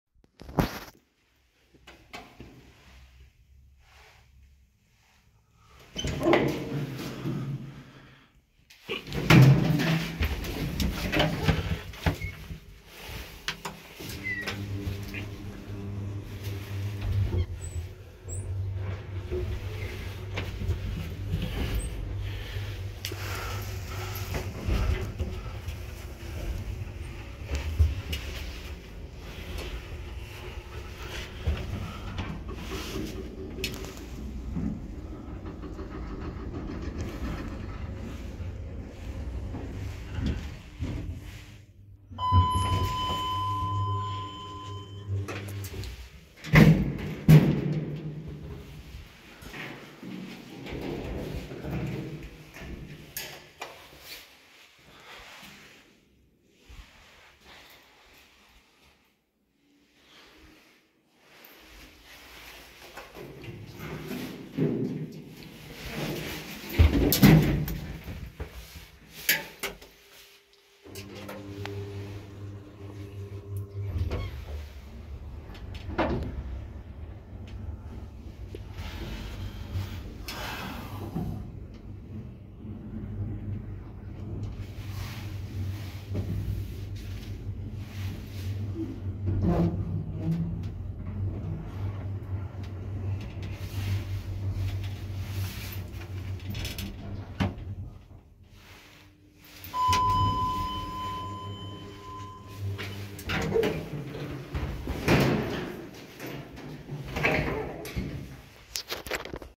Old IFMA traction elevator, mod. IFMA @ Bulevardul Unirii 44, Focșani, Romania
Technical Informations:
Manufacturer: IFMA
Model: T142
Year: 1970s
Type: Traction
Stops: 9 (P,1,2,3,4,5,6,7,8)
Capacity: 4 persons or 320 kg

Filmed in 13 November 2023, filmed with Samsung Galaxy A53 5G